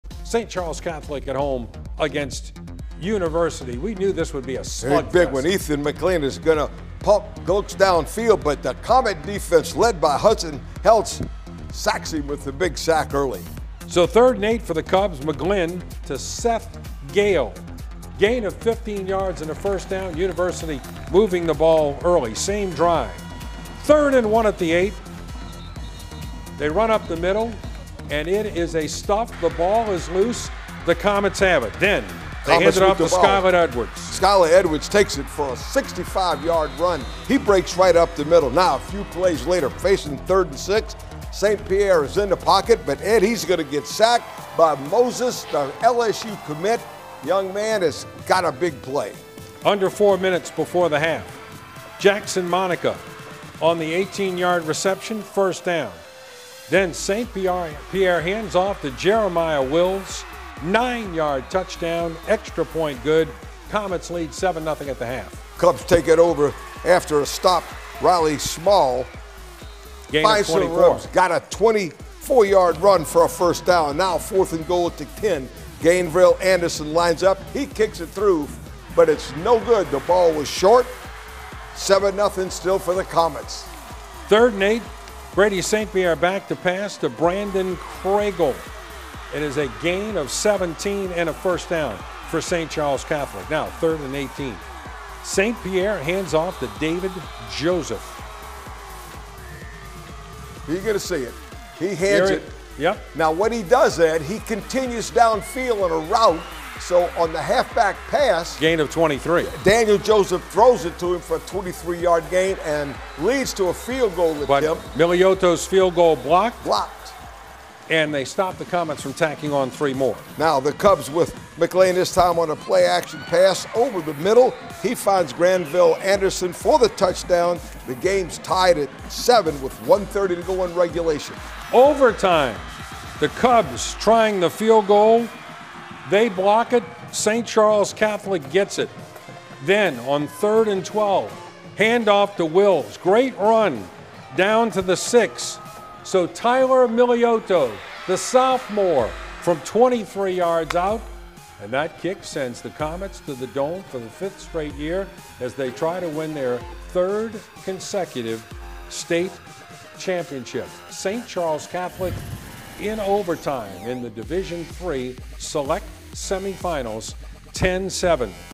FNF Semi-Finals: St. Charles Catholic wins 10-7 overtime thriller over University Lab
Tonight in Laplace, a Division III Select semi-final battle between (4) University Lab and (1) St. Charles Catholic.